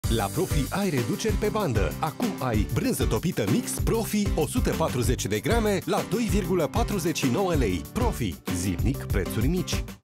La Profi ai reduceri pe bandă! Acum ai Brânză topită mix Profi 140g la un pret special.
La Profi ai reduceri pe bandă! Acum ai Brânză topită mix Profi 140g la un pret special. Oferta supusa unor termene si conditii. Profi, zilnic preturi mici.